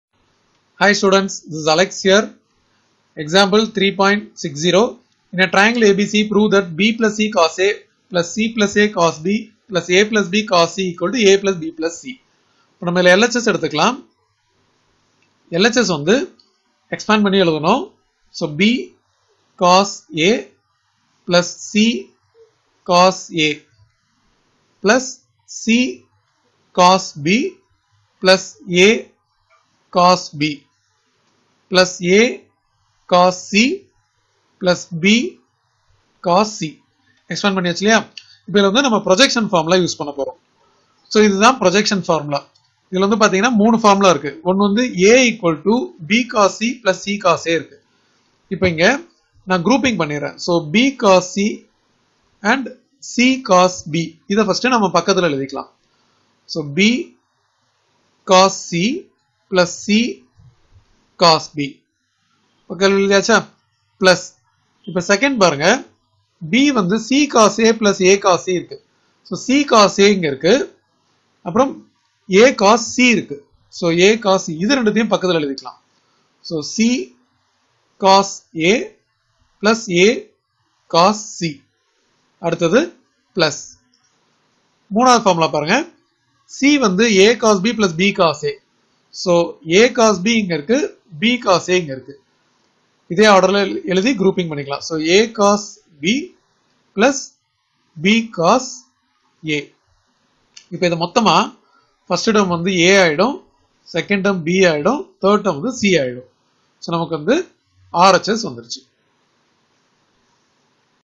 TN Class Class 11 Maths | Example 3.60 Trigonometry Chapter 3 | TN New Syllabus | AlexMaths
11th Maths Trigonometry New Syllabus Tamil Nadu Example 3.59

In a ABC, prove that (b + c) cosA + (c + a) cosB + (a + b) cosC = a + b + c

Properties of Triangle,The Law of Sines or Sine FormulaTrigonometric equations,Napier’s Formula
Law of Cosines,Projection Formula,Half-Angle formula,Area of a triangle (Heron’s Formula )